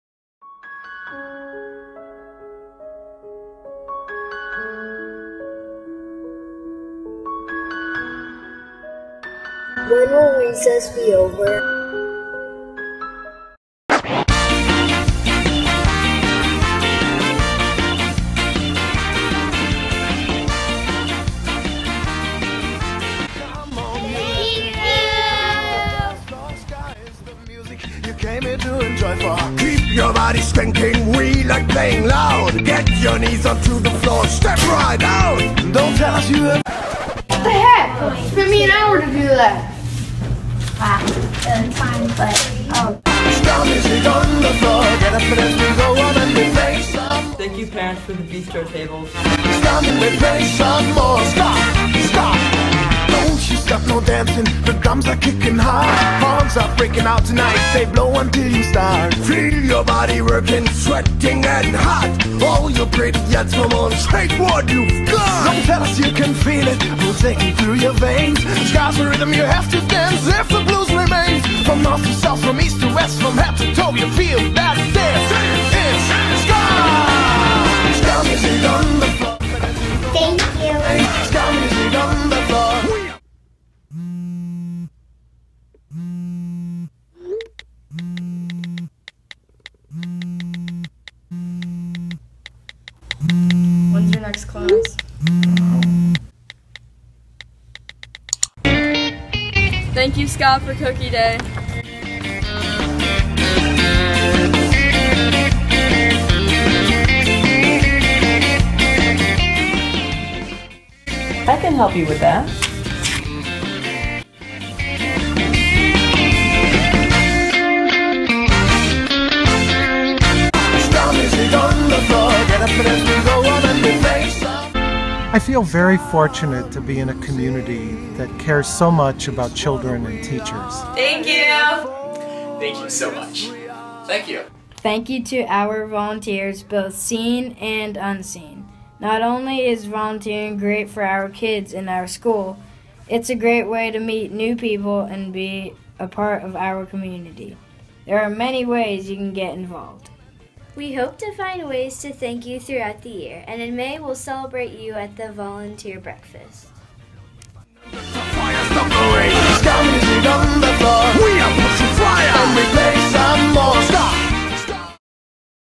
Thank you Shorecrest Volunteers!
See how the generous volunteers of the Shorecrest Preparatory School community have helped us grow! Also: The Shorecrest Parent Association (SPA) is now the Shorecrest Community Association (SCA)!